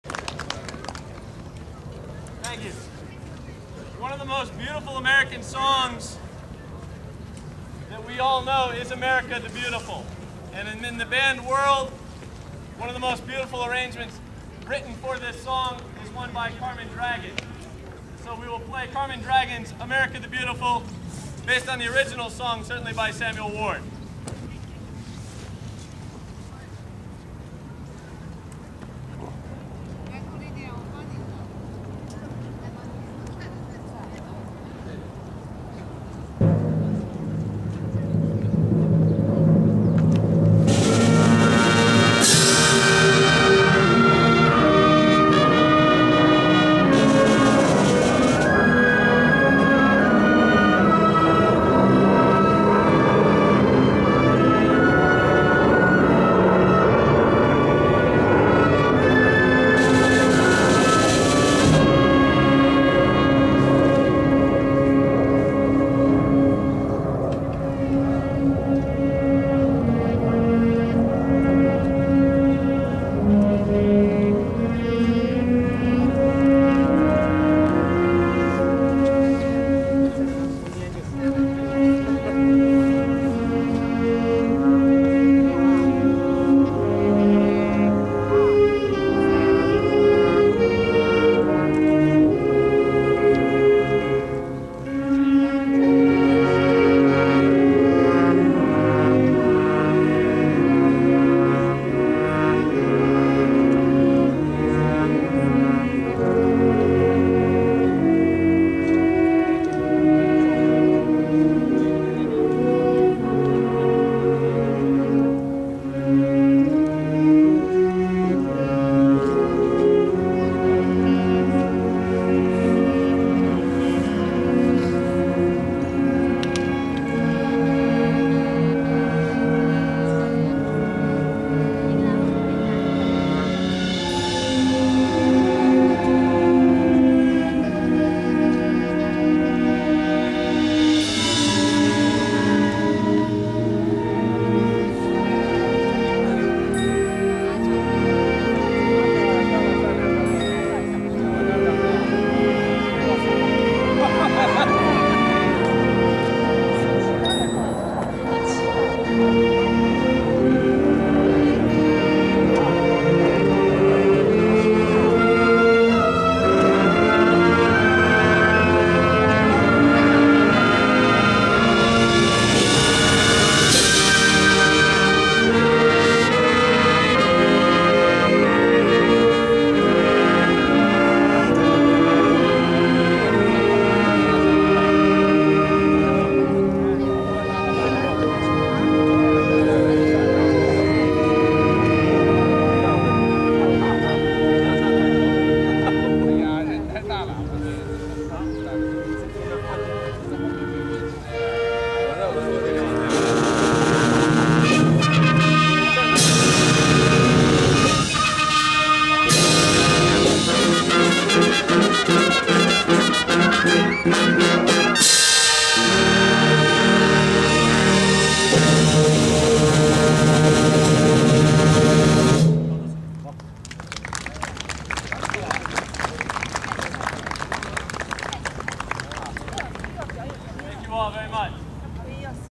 Central Bucks East Marching Band
Central Bucks East at The World War II Memorial-March 14 th 2009  Washington D C